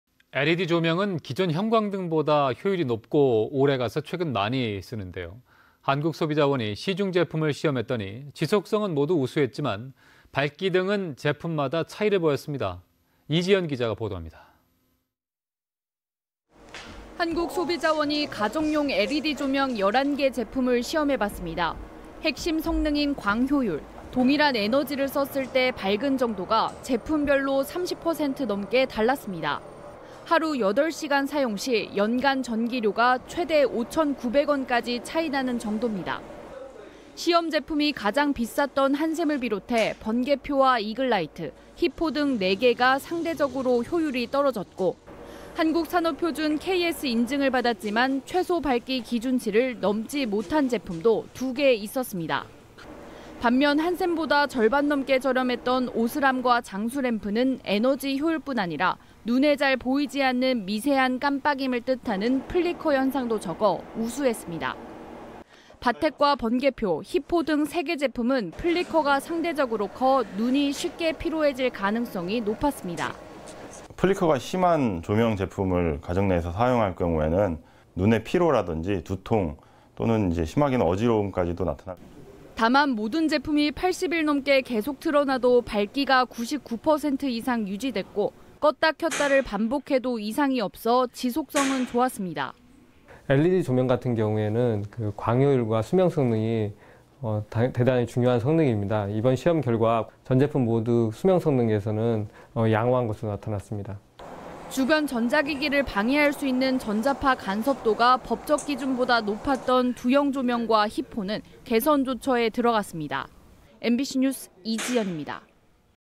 가정용 LED조명, 지속성 '우수' 밝기 '제각각'ㅣMBC충북NEWS
[앵커]
 LED 조명은 기존 형광등보다
효율이 높고 오래가 최근 많이 쓰는데요.

 한국소비자원이 시중 제품을 시험했더니
지속성은 모두 우수했지만,
밝기 등은 제품마다 차이를 보였습니다.

 이지현 기자가 보도합니다.

         [기자]
 한국소비자원이
가정용 LED조명 11개 제품을 시험해봤습니다.

 핵심 성능인 광 효율,
동일한 에너지를 썼을 때 밝은 정도가
제품별로 30% 넘게 달랐습니다.

 하루 8시간 사용 시 연간 전기료가
최대 5천9백 원까지 차이 나는 정도입니다.

(PIP1) 시험 제품이 가장 비쌌던 한샘을 비롯해
번개표와 이글라이트, 히포 등 4개가
상대적으로 효율이 떨어졌고,

(PIP2) 한국산업표준 KS인증을 받았지만
최소 밝기 기준치를 넘지 못한 제품도
2개 있었습니다.

(PIP3) 반면 한샘보다 절반 넘게 저렴했던
오스람과 장수램프는 에너지효율뿐 아니라
눈에 잘 보이지 않는 미세한 깜박임을 뜻하는
'플리커' 현상도 적어 '우수'했습니다.

(PIP4) 바텍과 번개표, 히포 등 3개 제품은
플리커가 상대적으로 커
눈이 쉽게 피로해질 가능성이 높았습니다.

   [ 공균용 선임연구원]
"플리커가 심한 조명 제품을 가정에서 사용할 경우에는 눈에 피로라든지 두통, 또는 심하게는 어지러움까지도 나타나는..."

 다만 모든 제품이 80일 넘게 계속 틀어놔도
밝기가 99% 이상 유지됐고,
껐다 켰다를 반복해도 이상이 없어
지속성은 좋았습니다.

   [ 양종철 팀장]
"LED 조명 같은 경우에는 광 효율과 수명 성능이 대단히 중요한 성능입니다. 이번 시험 결과, 전 제품 모두 수명 성능에서는 양호한 것으로 나타났습니다."

 주변 전자기기를 방해할 수 있는
전자파 간섭도가 법적 기준보다 높았던
두영조명과 히포는 개선 조처에 들어갔습니다.
MBC 뉴스 이지현입니다.(영상취재 천교화)